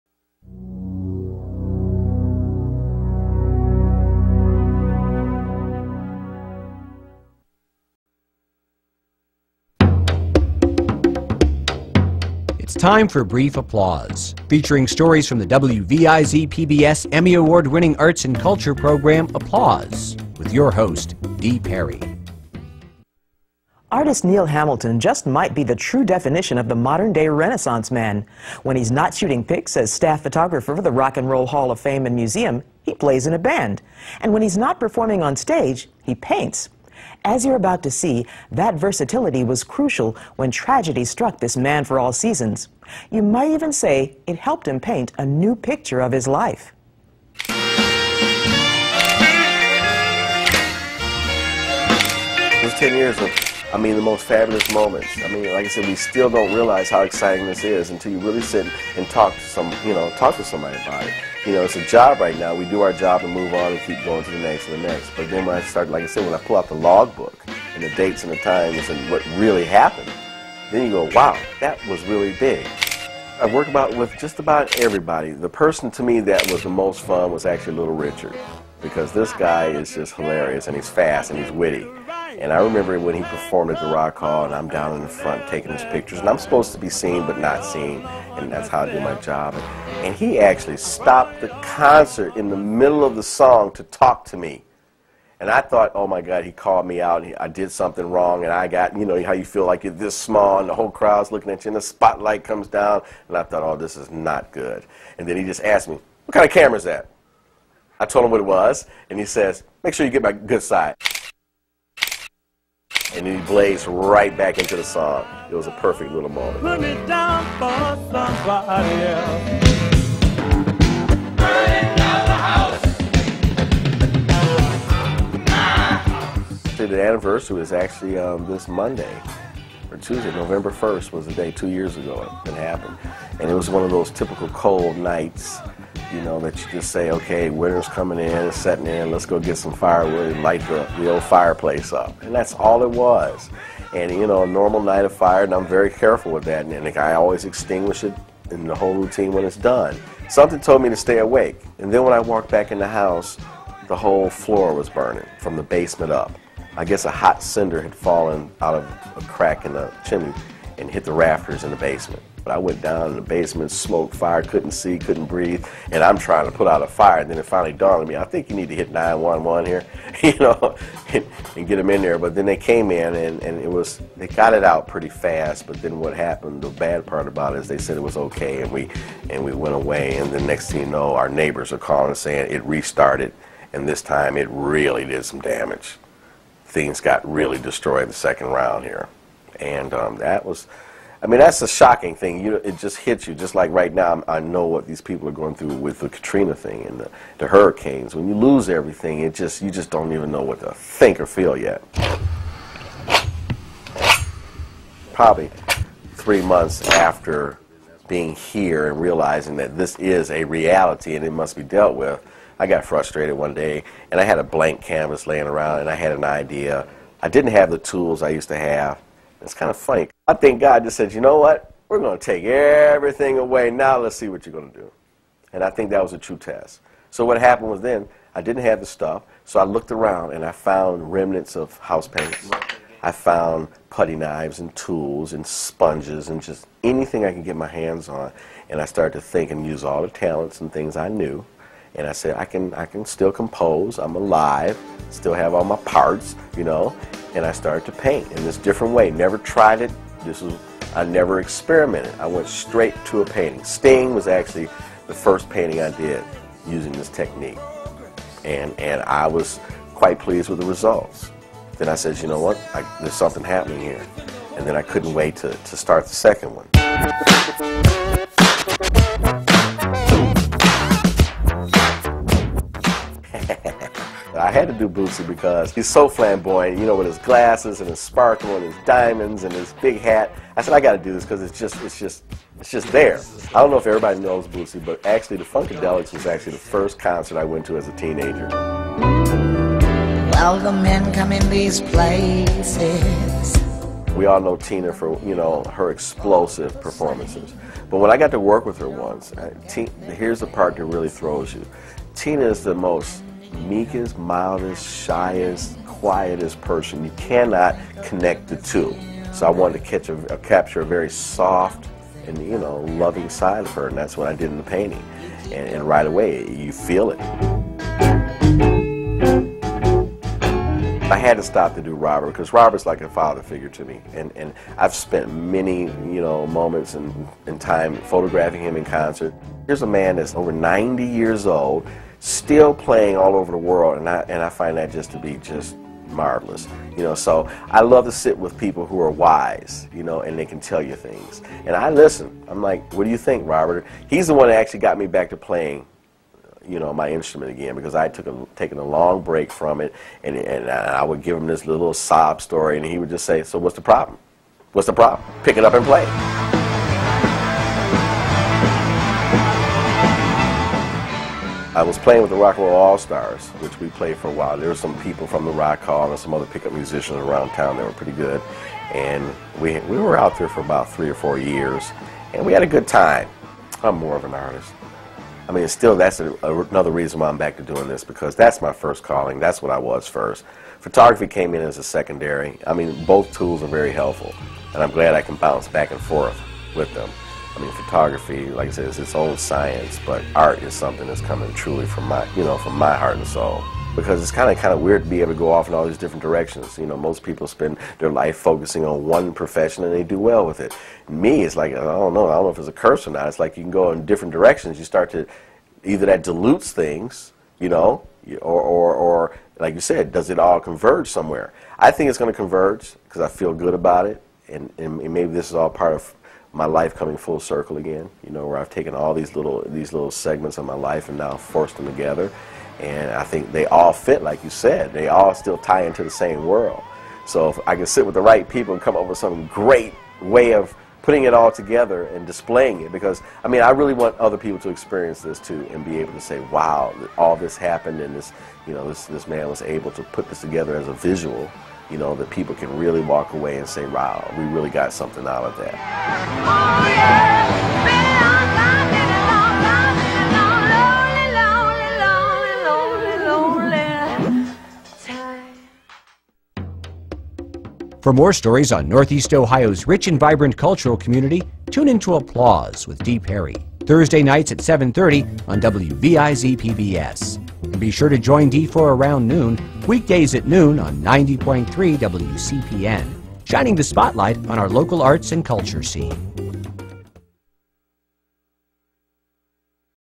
Brief Applause: Artist Neal Hamilton
Artist Neal Hamilton just might be the true definition of the modern day renaissance man: when he's not shooting pics as staff photographer for the Rock and Roll Hall of Fame and Museum, he plays in a band. And when he's not performing on stage, he paints. That versatility was crucial when tragedy struck this man. You might even say it helped him paint a new picture of his life.